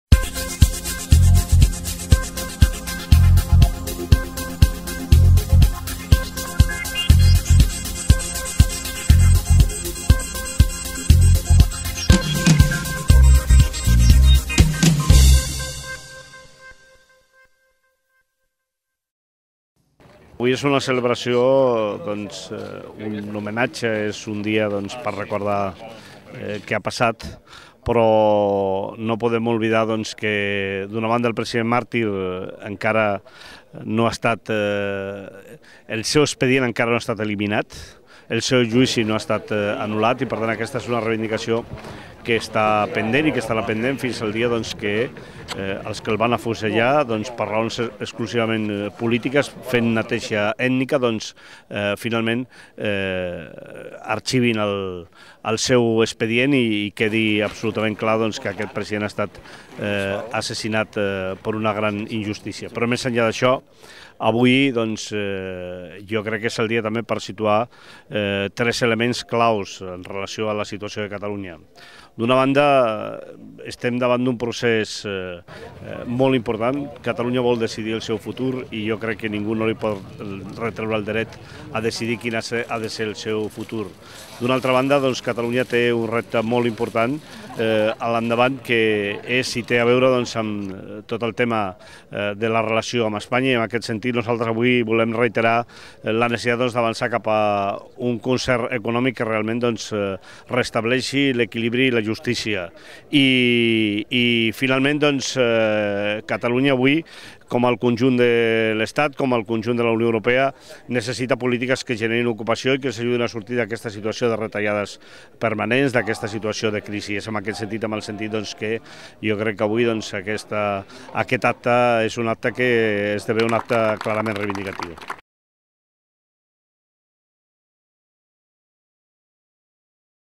Ofrena floral president Companys
Una delegació de la UGT de Catalunya, amb el seu secretari general al capdavant, ha participat avui en l'ofrena floral al president Lluís Companys, en l'aniversari del seu afusellament a Montjuïc.